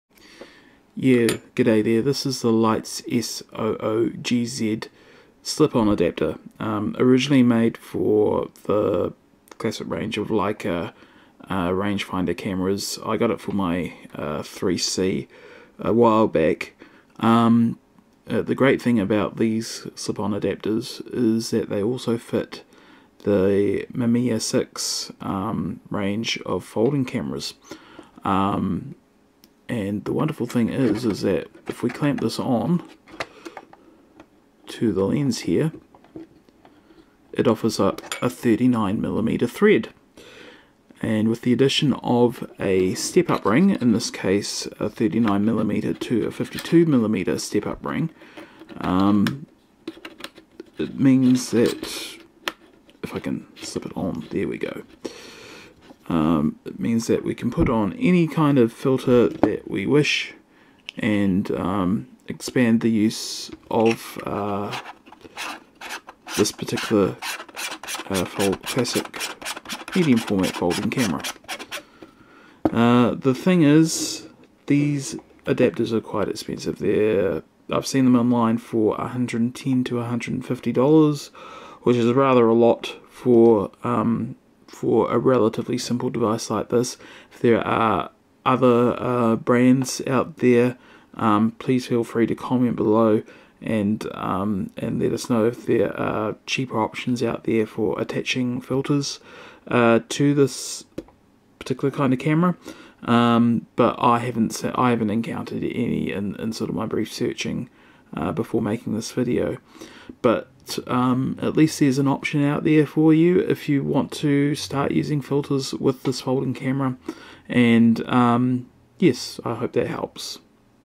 Attaching lens filters to the Mamiya Six folding camera
Using the Leitz SOOGZ slip on adapter to attach filters to the Mamiya Six folding camera lens